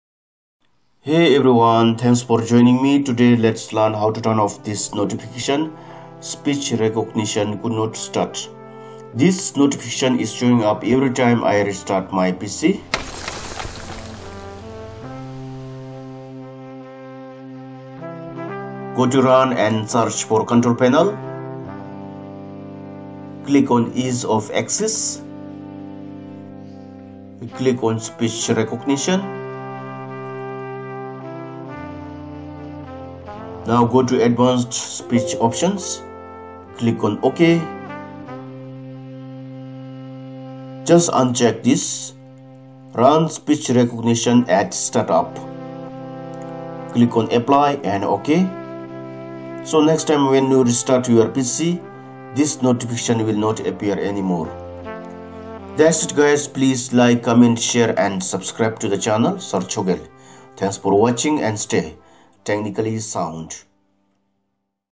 How To Turn Off Speech Recognition could not start
I got this notification every time I startup my PC, "Speech Recognition Error.
Speech Recognition could not start
Make sure your audio hardware is working properly and check your audio configuration in the Audio Devices and Sound Themes control panel."

Speech recognition can only support if your is PC connected to the mike. For example, a laptop can support speech recognition as it has an inbuilt mike system.

Don't worry. We can turn off the notification as shown in this video.